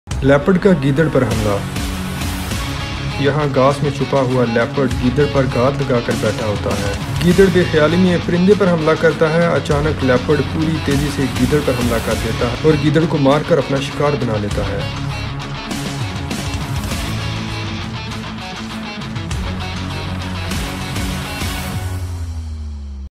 Leopard Vs Jackal - Leopard Attacks Jackal - 1 of 3 Most Incredible Leopard Attacks on Jackal
10 Times When Leopard Incredible Hunt Its Prey

Source:@Latestsightings